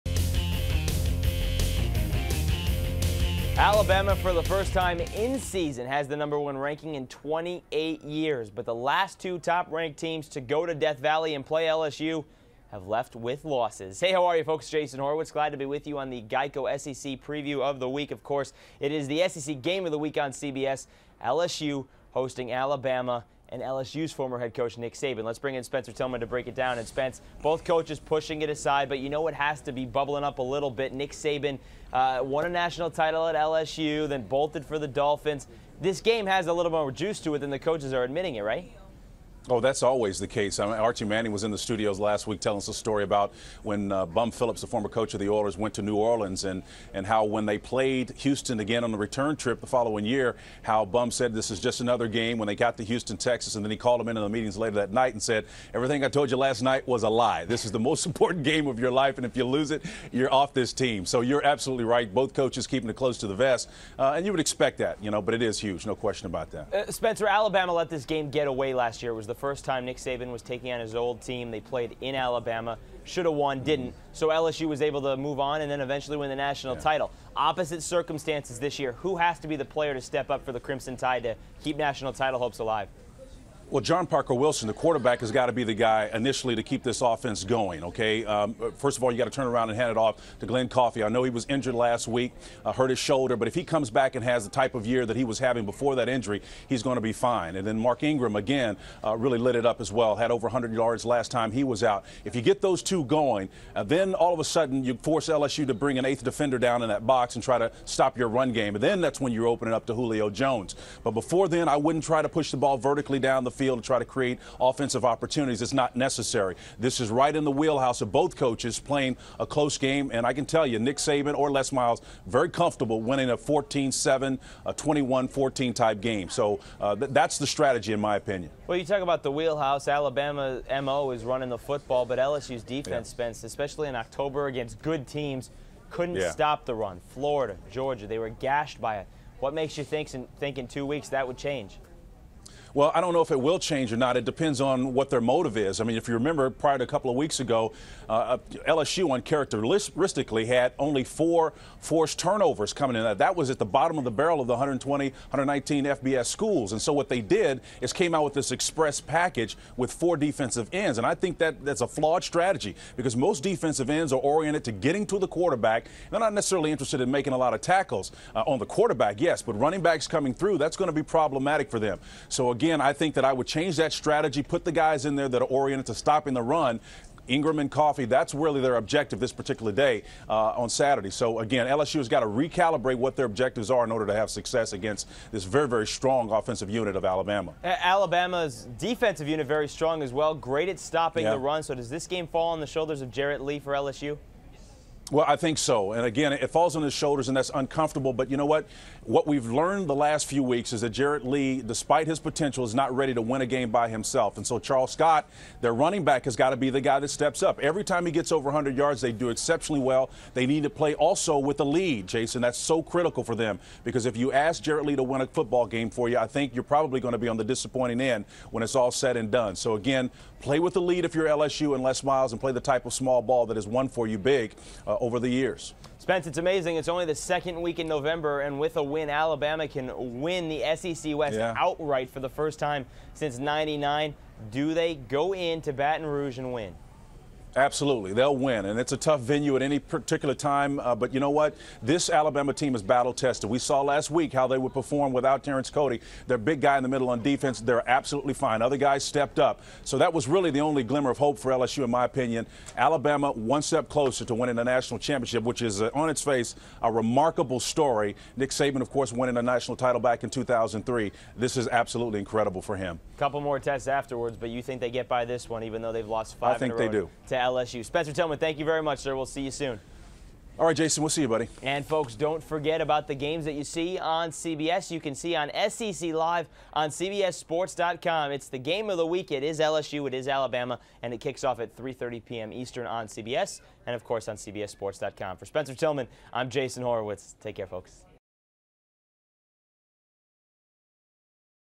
GEICO: Alabama vs. LSU
The top-ranked Crimson Tide will try to clinch the West Division title of the SEC and a spot in its championship game Saturday when they take on LSU. Jason Horowitz and Spencer Tillman break down this SEC showdown.